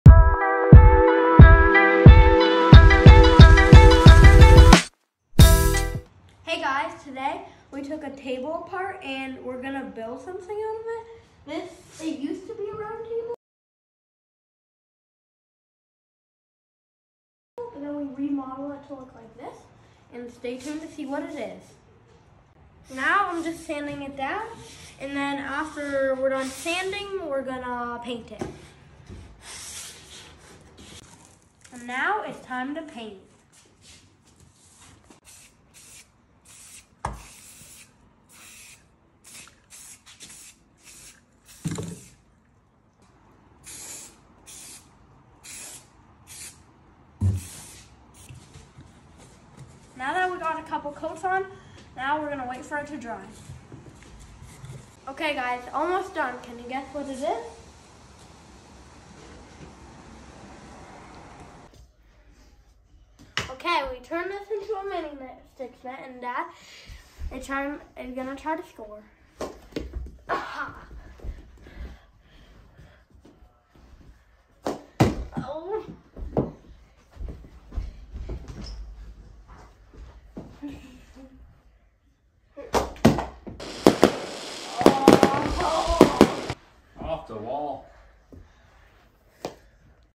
How to Build Your Own Mini Sticks Hockey Net With Hayden
In this video Hayden uses the scrapes of our round table(that is now a smaller square table) to build himself a new mini sticks hockey net. After a lot of hard work; sanding, painting, building and coming up with a creative design, Hayden has his own personalized mini sticks net.
He still has to practice his goalie technique as his dad schools him in the end!!! But Hayden is very happy with the end results.